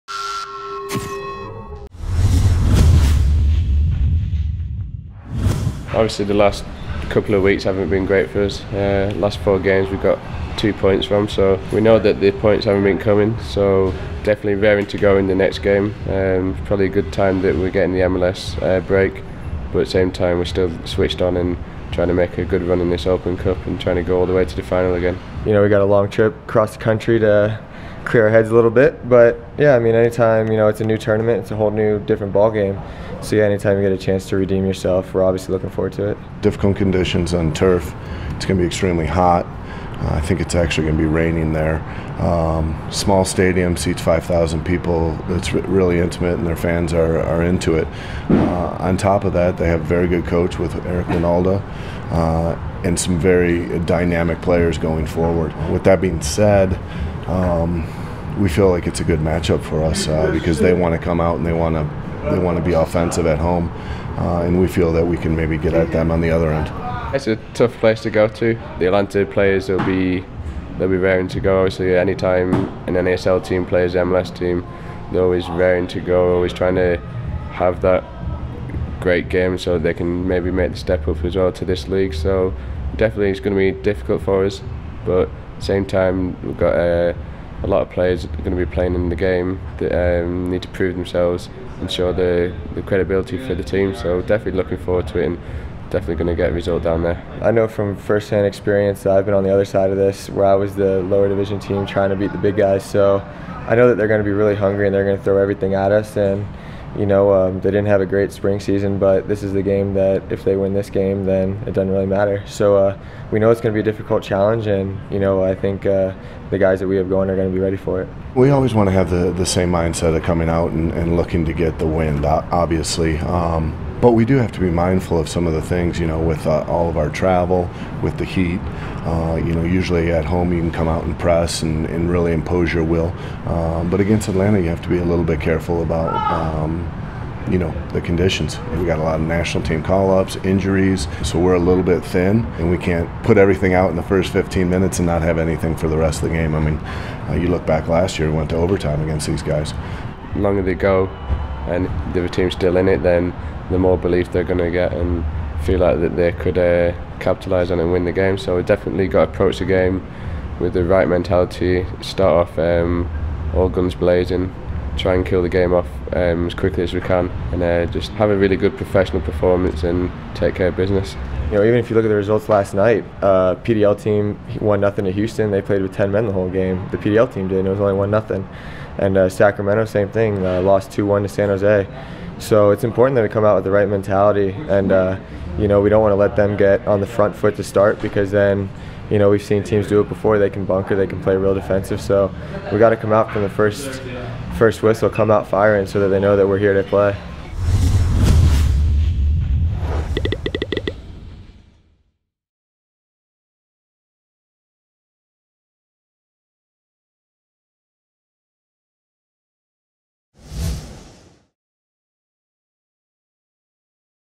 Real Salt Lake at Atlanta Silverbacks - U.S. Open Cup Match Preview
Real Salt Lake head coach Jeff Cassar, midfielder Luke Mulholland and goalkeeper Jeff Attinella preview tomorrow's U.S. Open Cup match against the Atlanta Silverbacks.